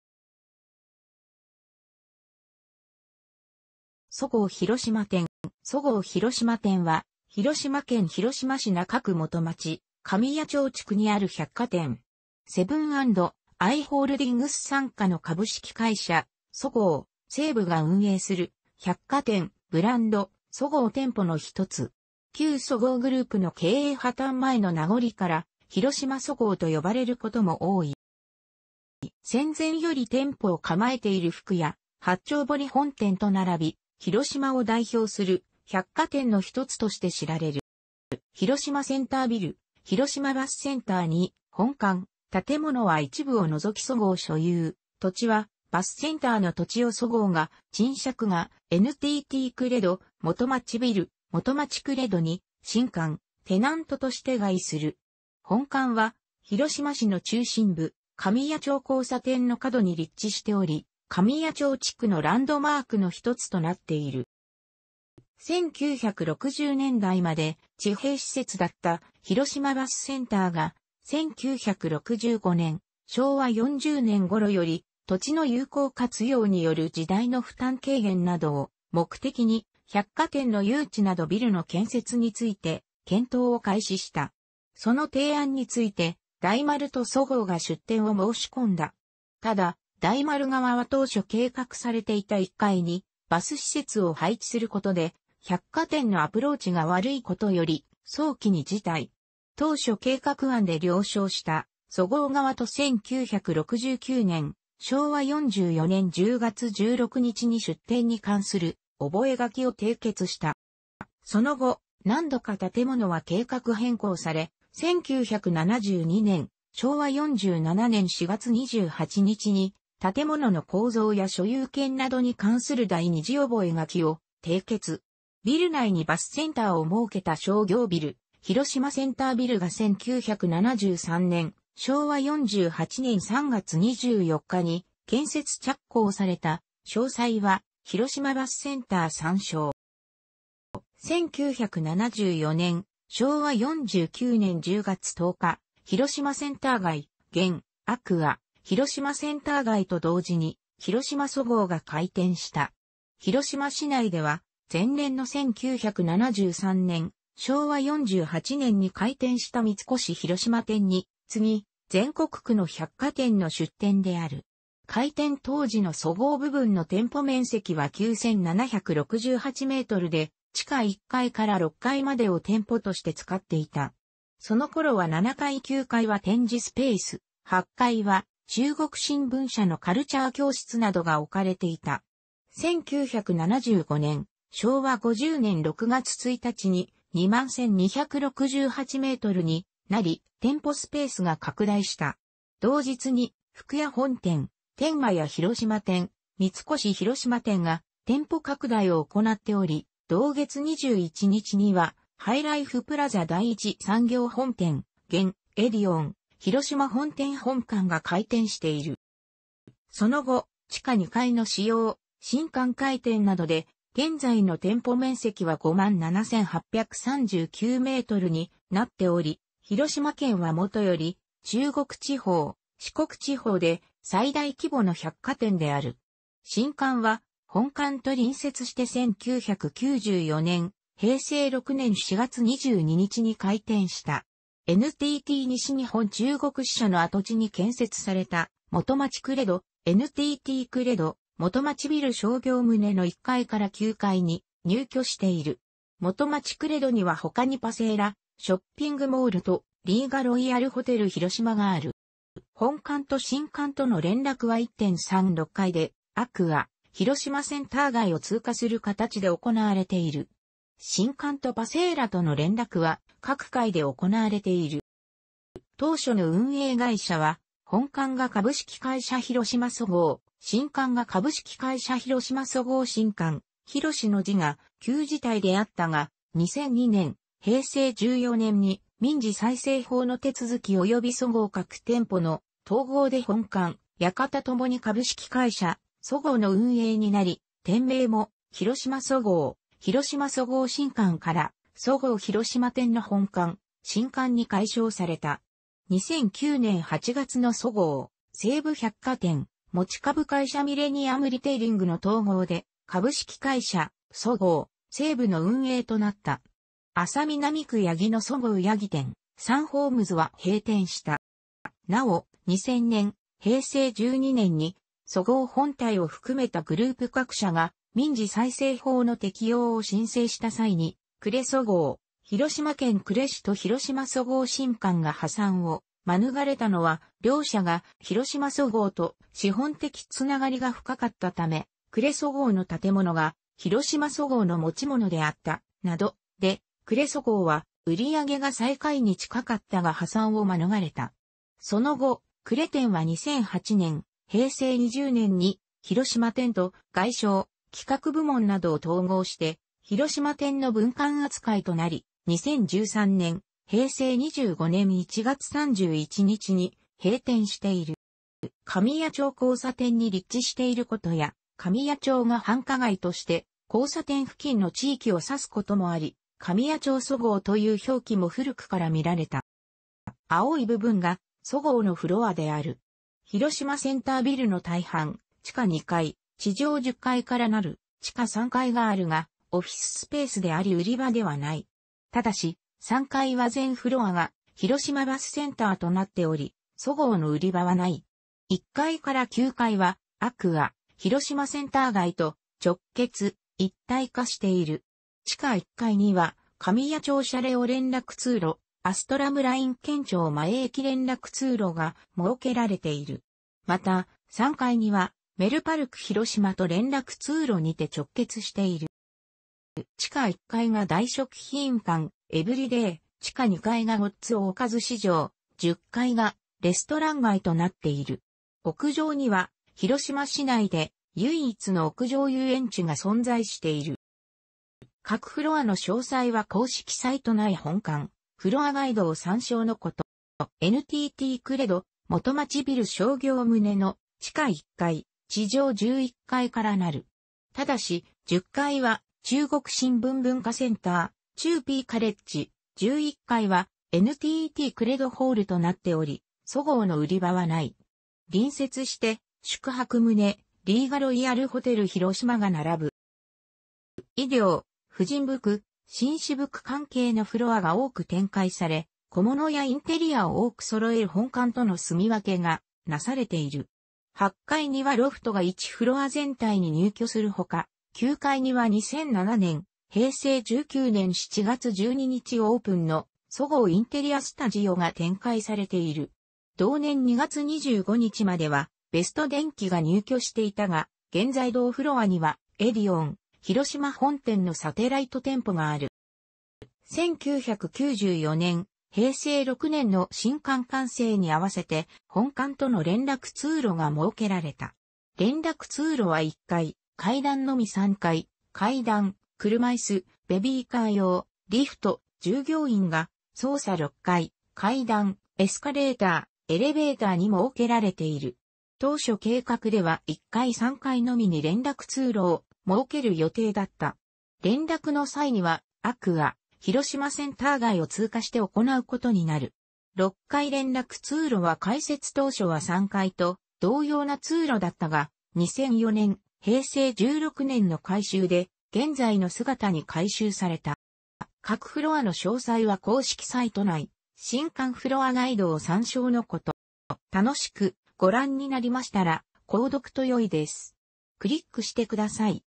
そごう広島店
そごう広島店（そごうひろしまてん）は、広島県広島市中区基町（紙屋町地区）にある百貨店。セブン&アイ・ホールディングス傘下の株式会社そごう・西武が運営する百貨店ブランド「そごう」の店舗の一つ。旧そごうグループの経営破綻前の名残から、広島そごうとよばれることも多い。

戦前より店舗を構えている福屋（八丁堀本店）と並び、広島を代表する百貨店の一つとして知られる。

広島センタービル（広島バスセンター）に本館（建物は一部を除きそごう所有・土地はバスセンターの土地をそごうが賃借）が、NTTクレド基町ビル（基町クレド）に新館（テナントとして）が入居する。本館は広島市の中心部・紙屋町交差点の角に立地しており、紙屋町地区のランドマークの一つとなっている。

1960年代まで地平施設だった広島バスセンターが、1965年（昭和40年）頃より土地の有効活用による地代の負担軽減などを目的に、百貨店の誘致などビルの建設について検討を開始した。その提案について、大丸とそごうが出店を申し込んだ。ただ、大丸側は当初計画されていた1階にバス施設を配置することで、百貨店のアプローチが悪いことより早期に辞退。当初計画案で了承した、そごう側と1969年（昭和44年）10月16日に出店に関する覚え書きを締結した。

その後、何度か建物は計画変更され、1972年（昭和47年）4月28日に、建物の構造や所有権などに関する第二次覚え書きを締結。ビル内にバスセンターを設けた商業ビル「広島センタービル」が1973年（昭和48年）3月24日に建設着工された（詳細は広島バスセンター参照）。

1974年（昭和49年）10月10日、「広島センター街」（現「アクア広島センター街」）と同時に「広島そごう」が開店した。広島市内では前年の1973年（昭和48年）に開店した三越広島店に次ぐ、全国区の百貨店の出店である。開店当時のそごう部分の店舗面積は9,768mで、地下1階から6階までを店舗として使っていた。その頃は7階・9階は展示スペース、8階は中国新聞社のカルチャー教室などが置かれていた。1975年（昭和50年）6月1日に、21,268mになり、店舗スペースが拡大した。同日に福屋本店・天満屋広島店・三越広島店が店舗拡大を行っており、同月21日にはハイライフプラザ第一産業本店（現・エディオン広島本店 本館）が開店している。

その後、地下2階の使用、新館開店などで現在の店舗面積は57,839mになっており、広島県はもとより中国地方・四国地方で最大規模の百貨店である。新館は本館と隣接して1994年（平成6年）4月22日に開店した。NTT西日本中国支社の跡地に建設された基町クレド（NTTクレド基町ビル）商業棟の1階 - 9階に入居している。基町クレドにはほかにパセーラ（ショッピングモール）とリーガロイヤルホテル広島がある。

本館と新館との連絡は1・3・6階でアクア広島センター街を通過する形で行われている。新館とパセーラとの連絡は各階で行われている。

当初の運営会社は本館が株式会社廣島そごう、新館が株式会社廣島そごう新館（「広」の字が旧字体）であったが、2002年（平成14年）に民事再生法の手続きおよびそごう各店舗の統合で本館・新館ともに株式会社そごうの運営になり、店名も「広島そごう」「広島そごう新館」から「そごう広島店」の本館・新館に改称された。2009年8月のそごう、西武百貨店、持ち株会社ミレニアムリテイリングの統合で株式会社そごう・西武の運営となった。安佐南区八木のそごう八木店（サンホームズ）は閉店した。

なお、2000年（平成12年）にそごう本体を含めたグループ各社が民事再生法の適用を申請した際に、呉そごう（広島県呉市）と広島そごう新館が破産を免れたのは、両者が廣島そごうと資本的つながりが深かったため（呉そごうの建物が、廣島そごうの持ち物であった、など）で、呉そごうは売り上げが最下位に近かったが破産を免れた。その後、呉店は2008年（平成20年）に広島店と外商・企画部門などを統合して広島店の分館扱いとなり、2013年（平成25年）1月31日に閉店している。

紙屋町交差点に立地していることや「紙屋町」が繁華街として交差点付近の地域を指すこともあり、「紙屋町そごう」という表記も古くから見られた。

※青い部分がそごうのフロアである。
広島センタービルの大半、地下2階・地上10階から成る（地下3階があるがオフィスス...